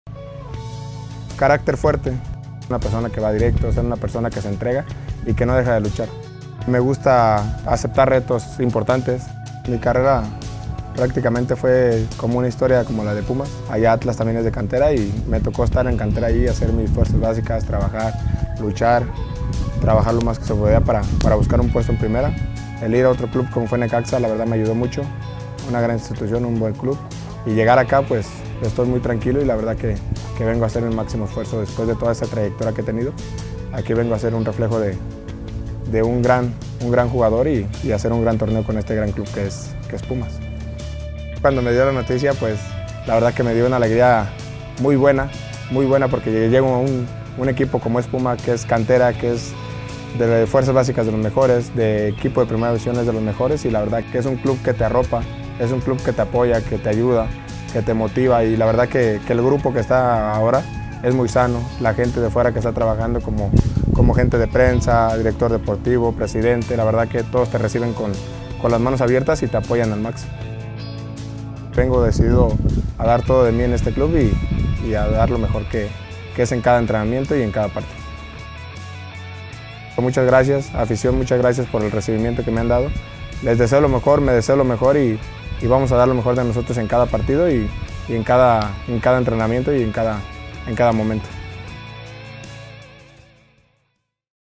Entrevista: Martín Barragán
Él es Martín Barragán, nuevo delantero de Universidad. Te compartimos la entrevista que le hicimos a su llegada a Pumas.